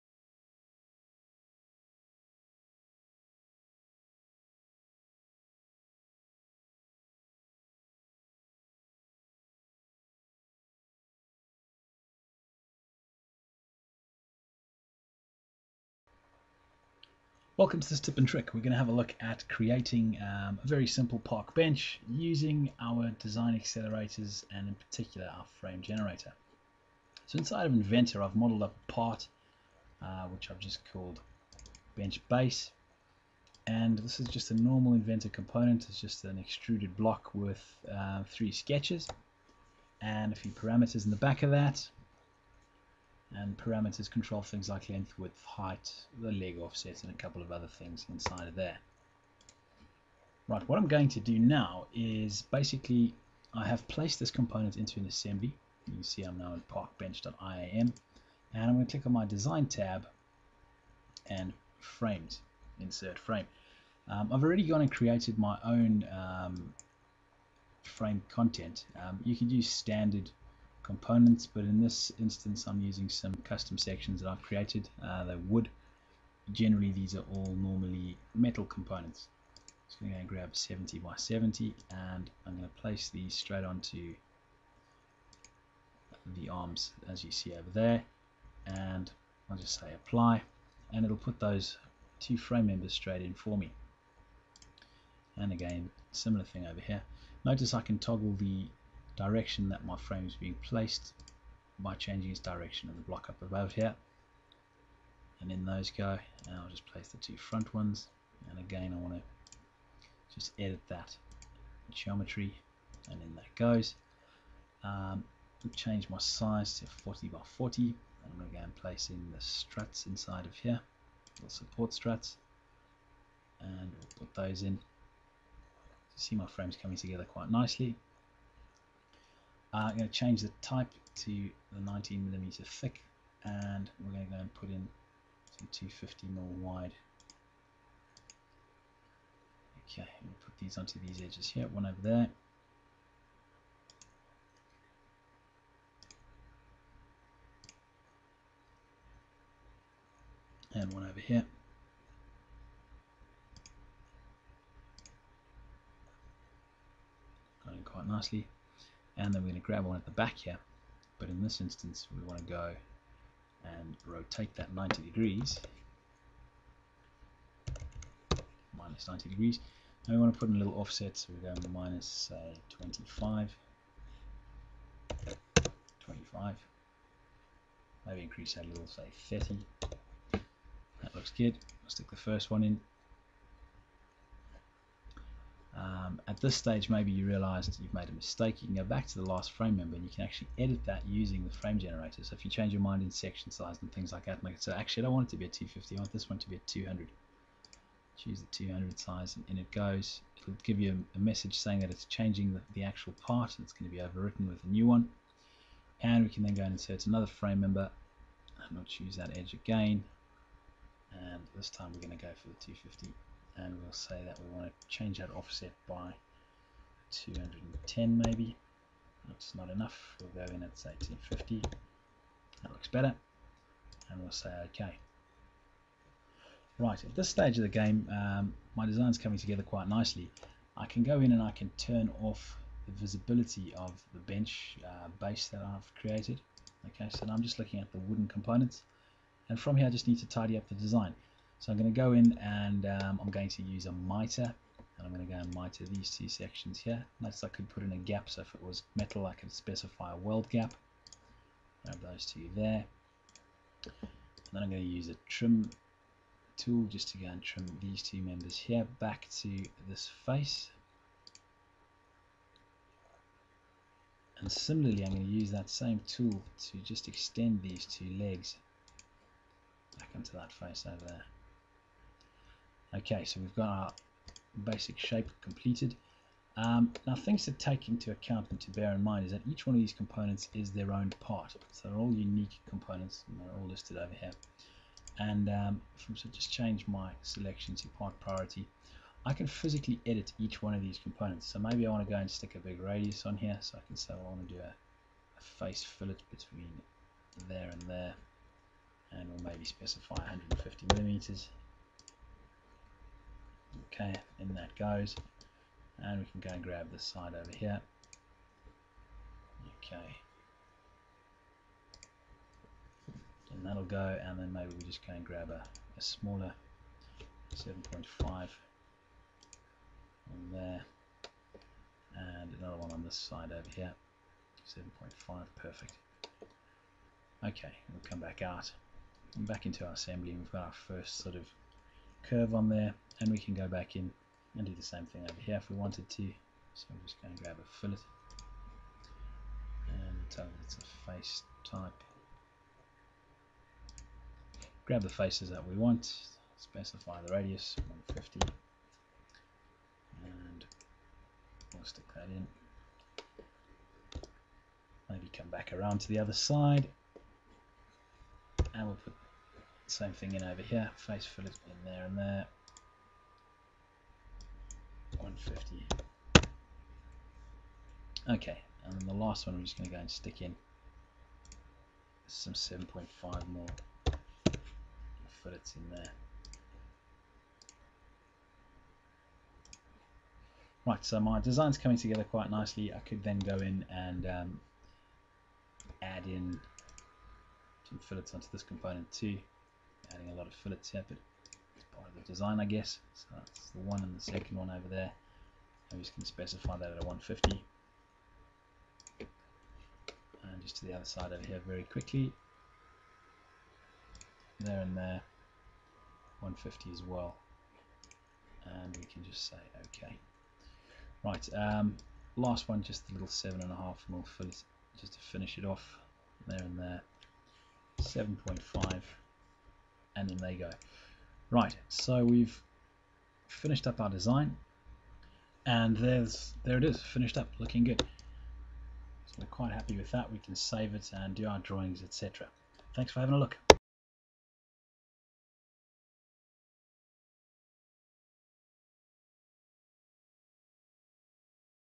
Midas Technology - AutoCAD Inventor Suite 2011 - Creating Frames in Inventor 2011
Midas Technology presents an AutoCAD Inventor Suite 2011 Technical Tip and Trick titled "AIS 026 - Creating Frames in Inventor 2011".